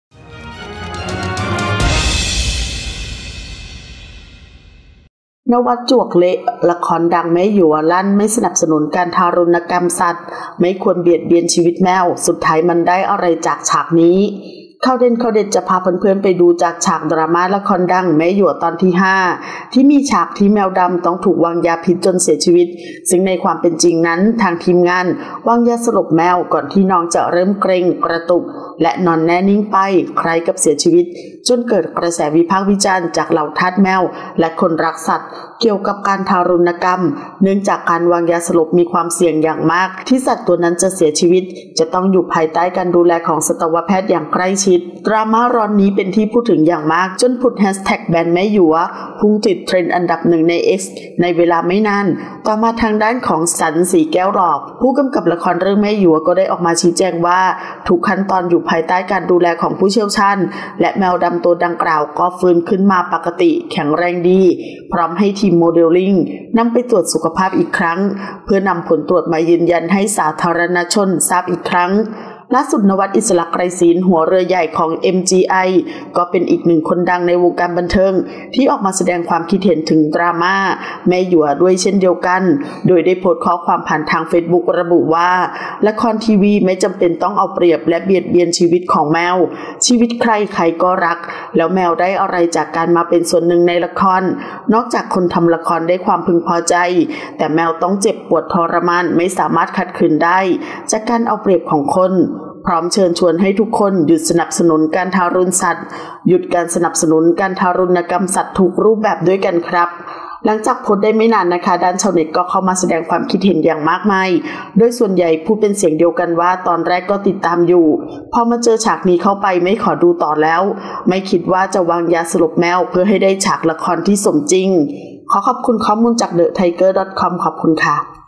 “ณวัฒน์” จวกเละ ละครดัง “แม่หยัว” ลั่น ไม่ควรเบียดเบียนชีวิตแมว สุดท้ายมันได้อะไรจากฉากนี้ : RECAP
“ณวัฒน์” จวกเละ ละครดัง “แม่หยัว” ลั่น ไม่สนับสนุนการทารุณสัตว์ ไม่ควรเบียดเบียนชีวิตแมว สุดท้ายมันได้อะไรจากฉากนี้ : RECAP

ขอขอบคุณข้อมูลจาก: thethaiger

00:00 - Intro
00:10 - News
01:00 - Minute